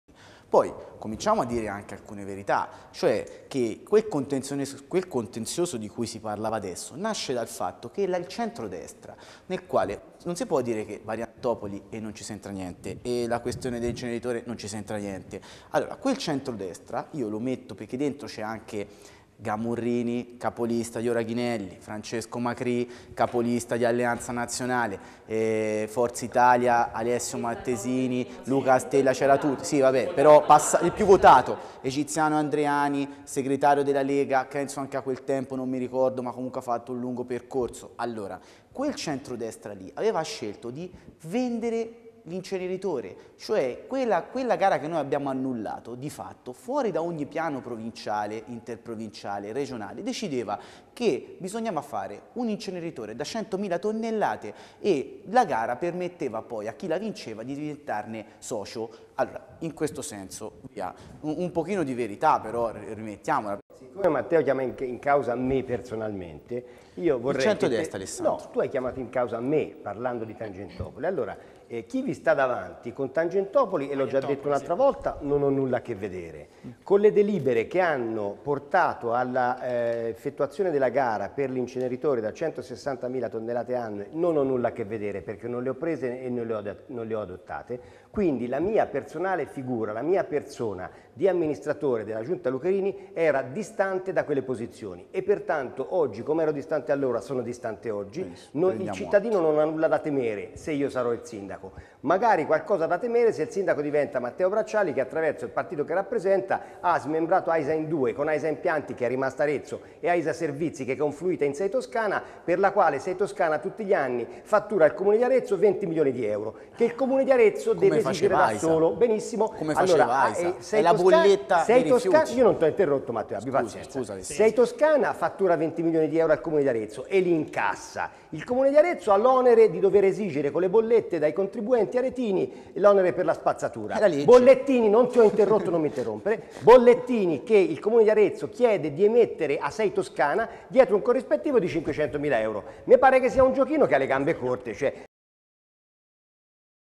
Bracciali-Ghinelli, l'ultimo confronto tv prima del voto
Alle 21,30 su Arezzo Tv (canale 214 del digitale terrestre) andrà in onda l'ultimo confronto tv tra i candidati a sindaco di Arezzo Matteo Bracciali e Alessandro Ghinelli. Un dibattito ancora acceso che si è di nuovo scaldato questa volta su Aisa, rifiuti e qualche altro rigurgito di Variantopoli.